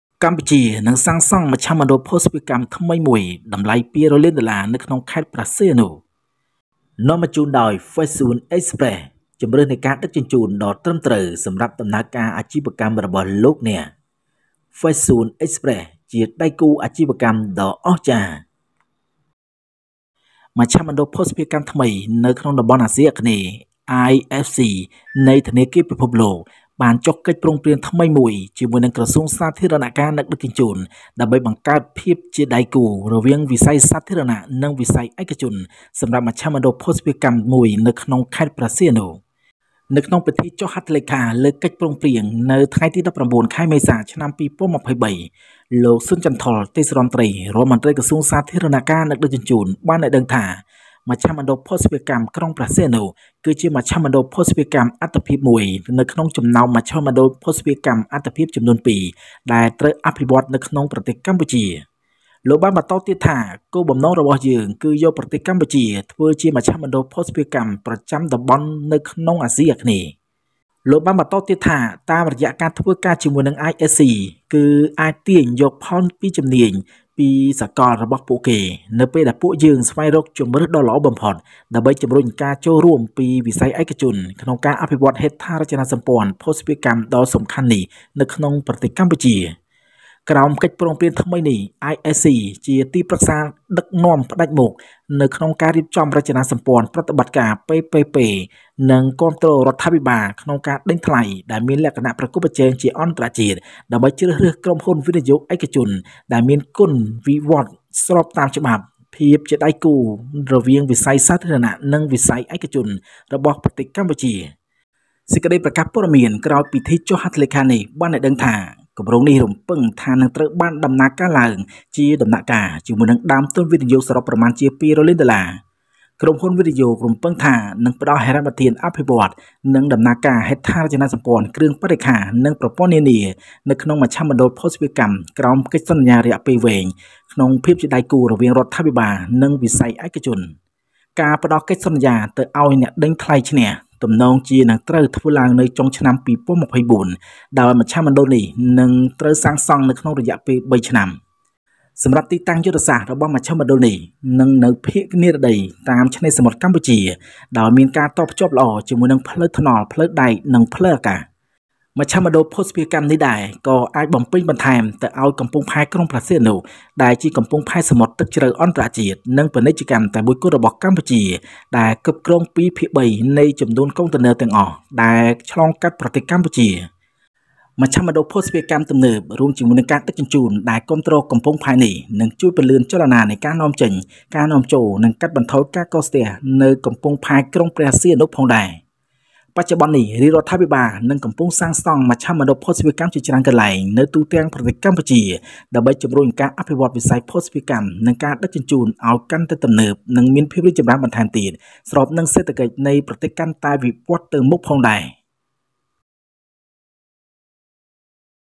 កម្ពុជានឹងសាងសង់មជ្ឈមណ្ឌលភស្តុភារកកម្មថ្មីមួយតម្លៃ២០០លានដុល្លារនៅខេត្តព្រះសីហនុ
The World Bank's New Center for Logistics in Southeast Asia (IFC) has signed a new agreement with the Ministry of Public Works and Transport (MPWT) to establish a public-private partnership for a logistics center in Sihanoukville. At the signing ceremony of the agreement on April 19, 2023, Mr. Sun Chanthol, Senior Minister of the Ministry of Public Works and Transport, said that the Sihanoukville Logistics Center is one of the priority logistics centers.  Two priorities to be developed in Cambodia.